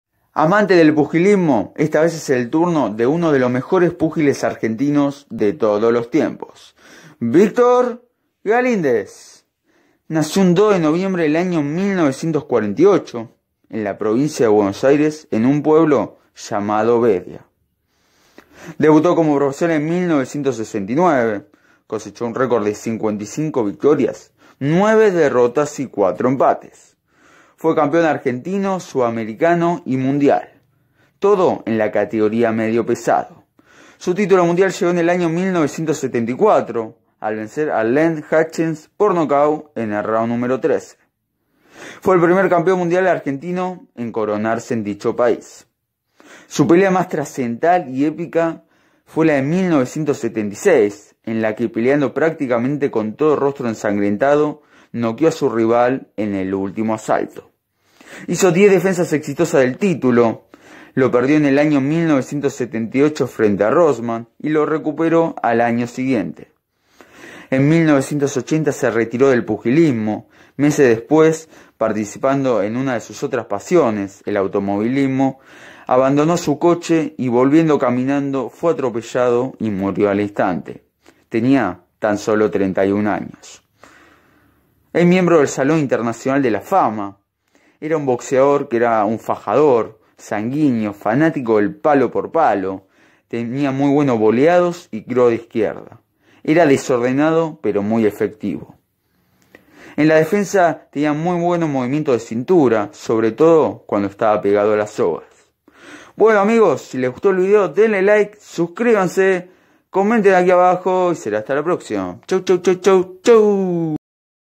El sanguíneo peleador Argentino que derrochaba guapeza
Fue campeón sudamericano, Ar
gentino y mundial .Protagonista de una de las peleas más emblemáticas de todos los tiempos y considerado uno de los mejores púgiles Argentinos.